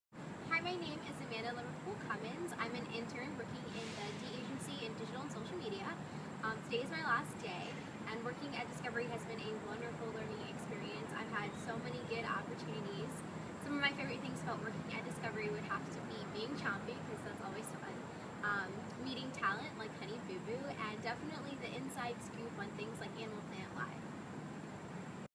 30 Seconds with...Amanda, A Discovery Intern
If we gave you thirty seconds to share something about your world , what would you say? In our video series, "30 Seconds with.." we'll introduce you to members of the Discovery Communications family and let you see how they define themselves in thirty seconds and what they're working on.